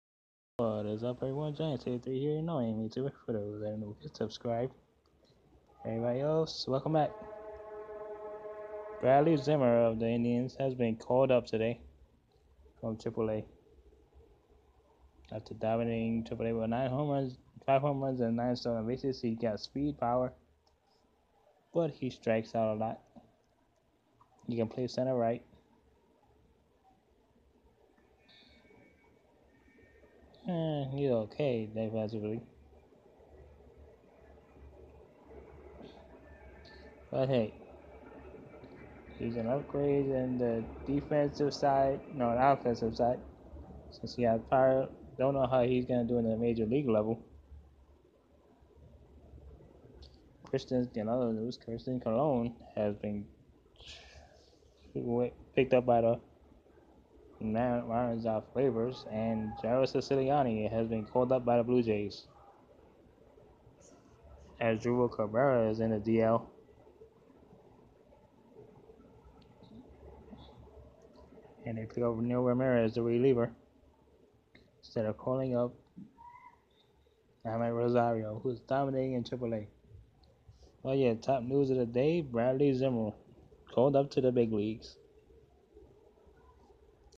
Indians call up prospect bradley zimmer
Indians call up zimmer blue jays with a call up few signings